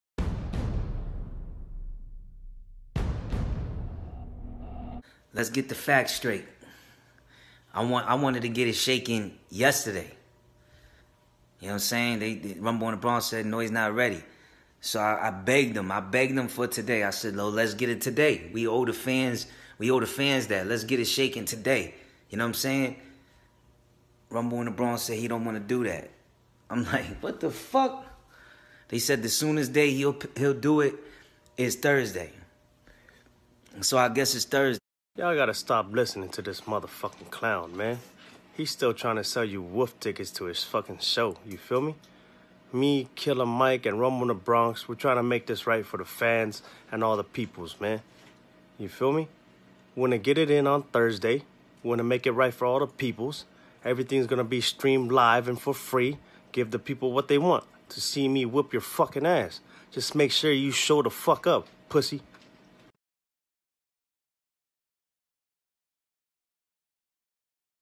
Jin Gates vs BUC NOI fight night Thursday !!!🥊🥊
Buc n Jin both agree on Thursday night to do the fight